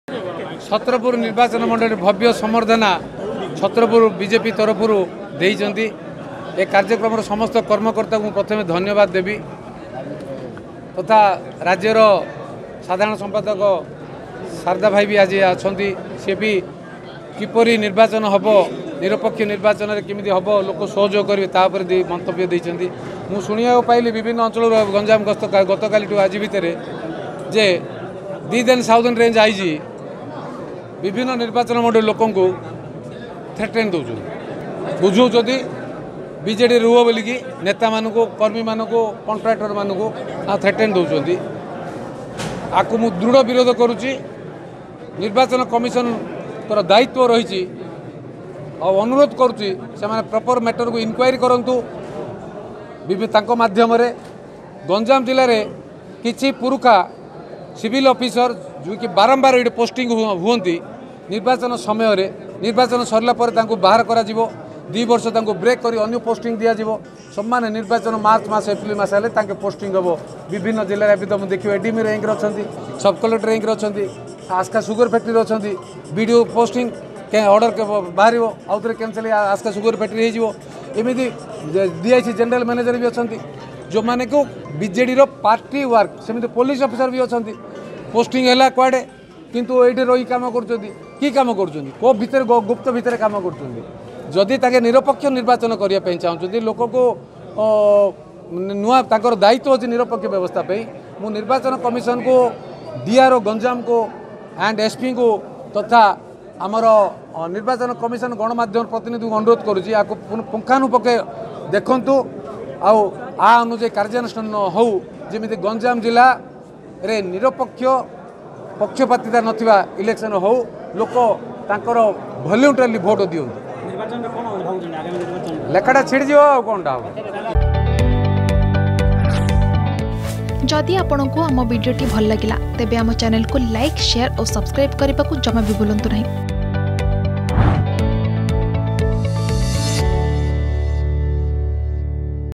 ଗଞ୍ଜାମରେ ନିରପେକ୍ଷ ନିର୍ବାଚନ ପାଇଁ ଆହ୍ଵାନ କଲେ ପ୍ରଦୀପ ପାଣିଗ୍ରାହୀ || Pradeep Panigrahi
ଗଞ୍ଜାମରେ ନିରପେକ୍ଷ ନିର୍ବାଚନ ପାଇଁ ଆହ୍ଵାନ କଲେ ବିଧାୟକ ପ୍ରଦୀପ ପାଣିଗ୍ରାହୀ। ଶୁଣନ୍ତୁ କ'ଣ କହିଲେ ।